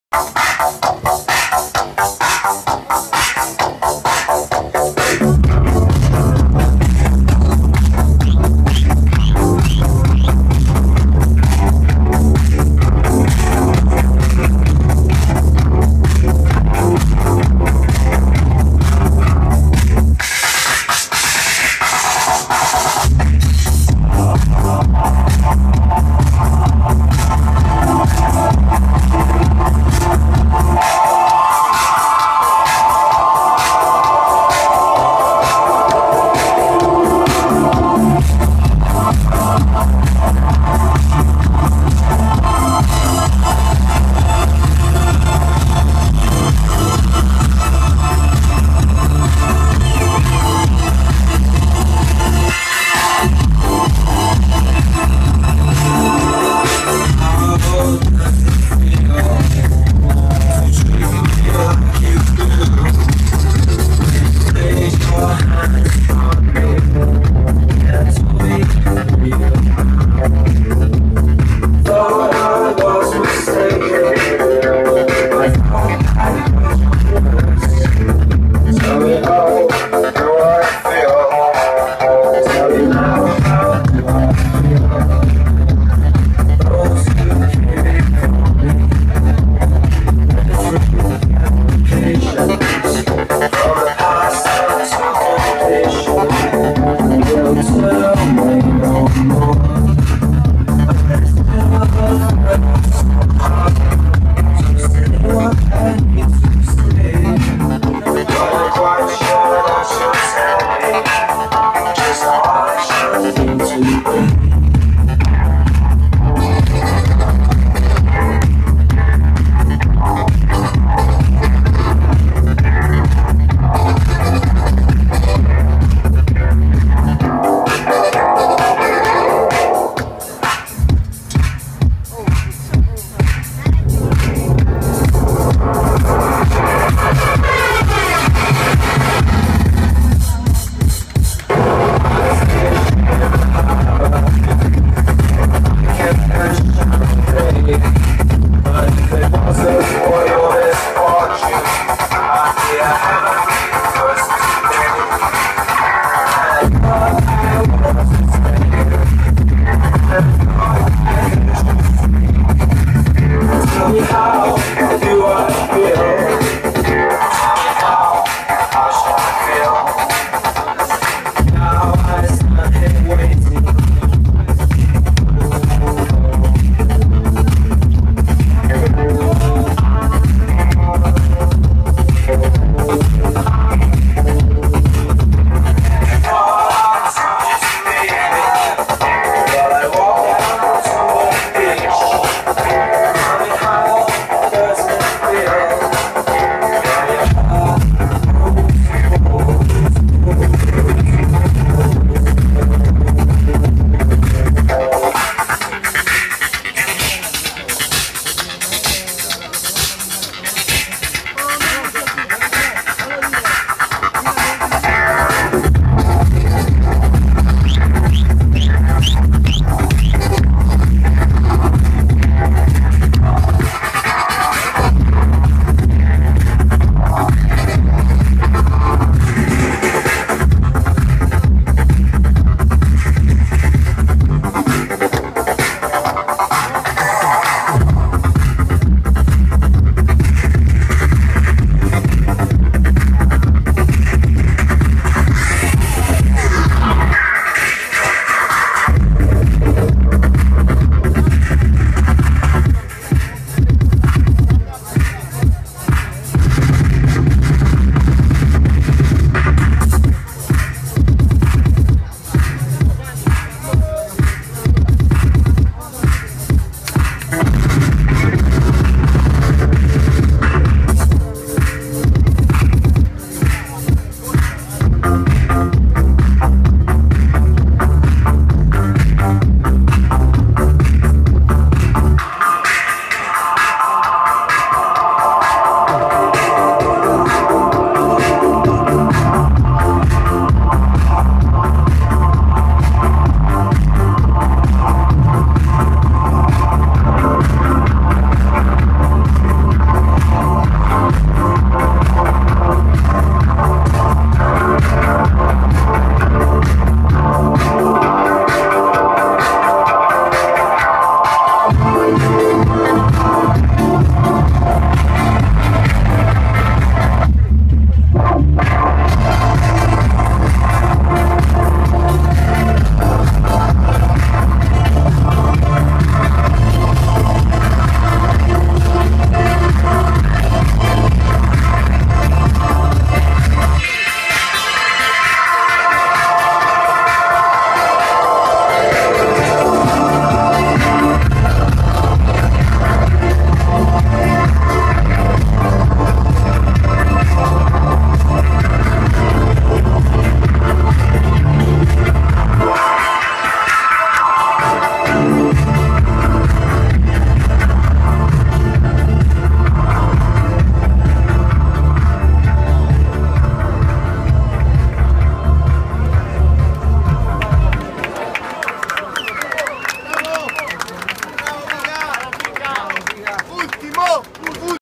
PICA DJ @DIRTY CLUB meets IBIZA TALENTS 06/02/15 MOSES CLUB
PICA DJ PLAY LAST TRACK AT MOSES CLUB 06/02/2015

DIRTY CLUB meets IBIZA TALENTS